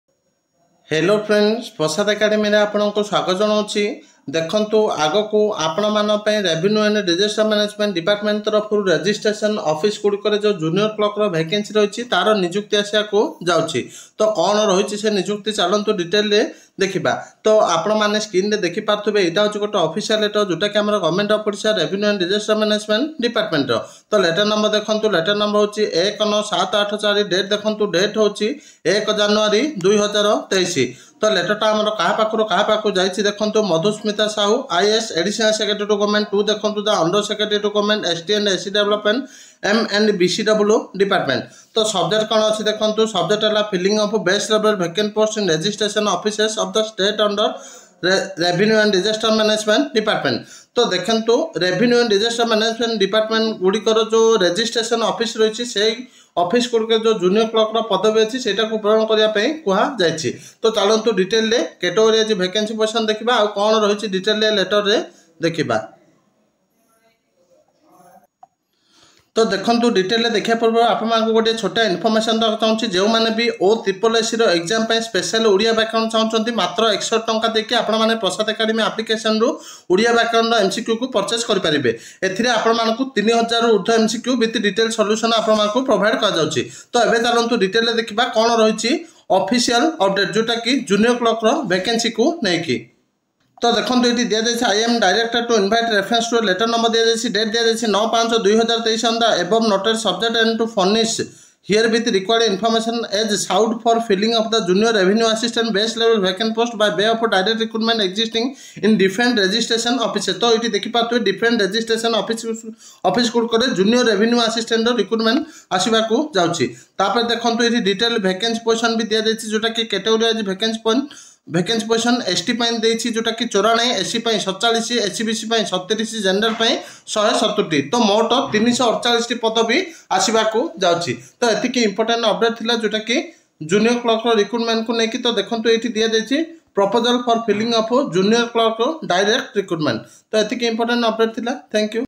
OSSSC/OSSC ରେ ଆସୁଛି ନିଯୁକ୍ତି || ଆସିବ 1200 ରୁ ଅଧିକ ନିଯୁକ୍ତି
download Application

visit website